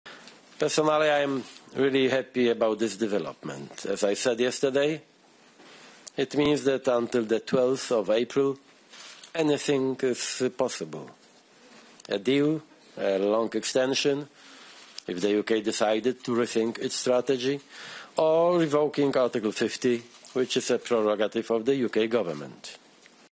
EU's Tusk: Anything is possible before April 12 Brexit day
EU Council President Donald Tusk says that until the new April 12 deadline, "anything is possible" on Brexit, including Britain cancelling its withdrawal. SOUNDBITE of Donald Tusk